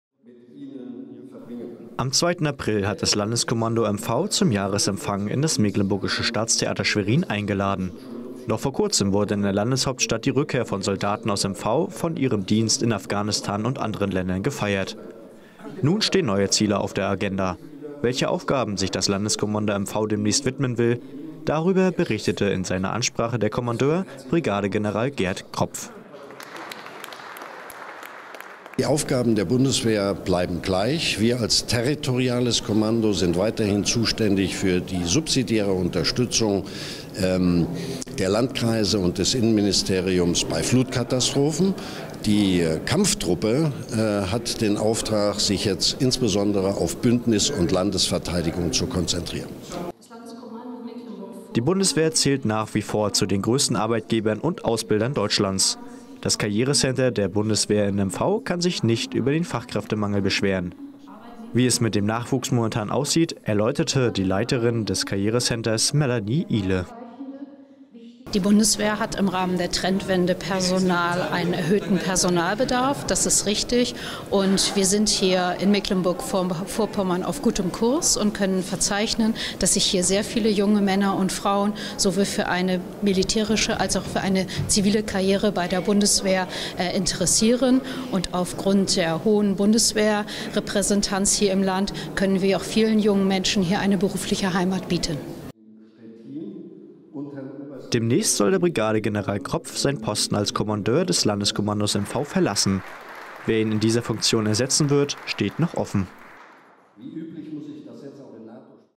Jahresempfang des Landeskommandos der Bundeswehr in Schwerin
Am 2. April hat das Landeskommando MV zum Jahresempfang in das Mecklenburgische Staatstheater Schwerin eingeladen. Noch vor Kurzem wurde in der Landeshauptstadt die Rückkehr von Soldaten aus MV von ihrem Dienst in Afghanistan und anderen Ländern gefeiert. Nun stehen neue Ziele auf der Agenda.